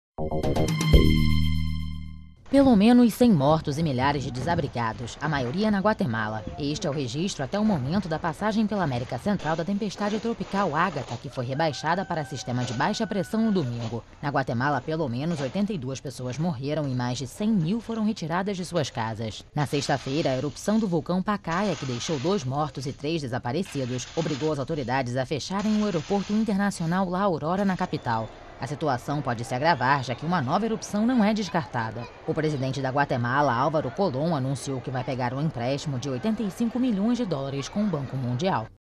Guatemala sofre com tempestade tropical e vulcão
Os guatemaltecos suportaram no sábado os efeitos da primeira tempestade tropical da temporada, Agatha - que virou depressão -, e da violenta erupção do vulcão Pacaya, que até o momento causaram a morte de pelo menos 84 pessoas.